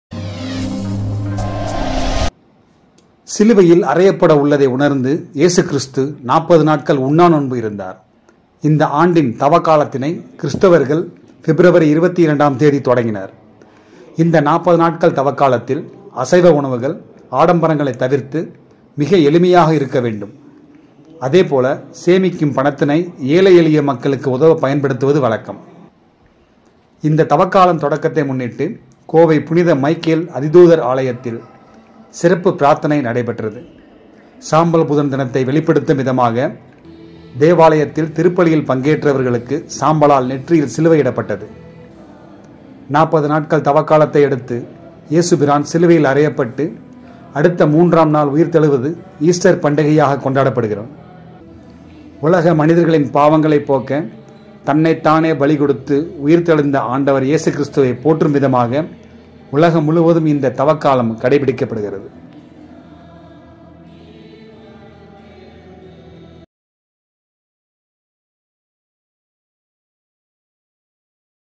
சாம்பல் புதன்!என்ன செய்ய வேண்டும்?ash Wednesday
ஏசு கிறிஸ்துவை சிலுவையில் அறையப்பட்டதை நினைவு கூறும் விதமாக உலகம் முழுவதும் உள்ள கிறிஸ்தவர்கள் 40 நாட்கள் நோன்பிருந்து, வறியவர்களுக்கு உதவிகள் செய்வது வழக்கம். இந்த நாட்களை அவர்கள் ஆண்டுதோறும் தவக்காலமாக கடைபிடித்து வருகிறார்கள்.தூய மைக்கேல் பேராலயத்தில் திருப்பலி ஆராதனை நடைபெற்று அனைவரின் நெற்றியிலும் சாம்பல் பூசப்பட்டது.